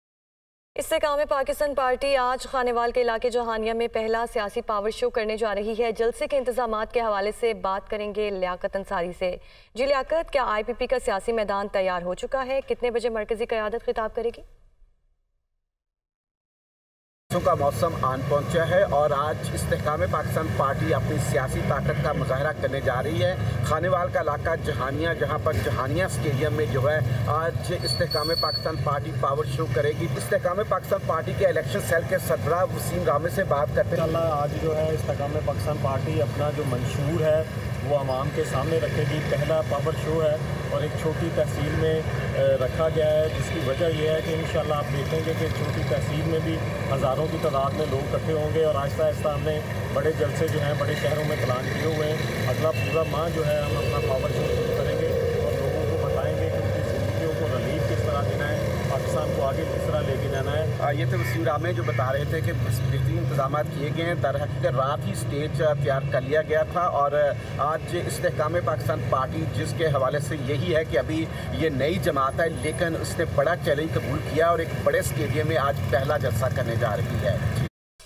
IPP Grand Powershow Khanewal Prep Update! Dunya News
In this video, we bring you the latest updates on the IPP Grand Powershow preparation in Khanewal.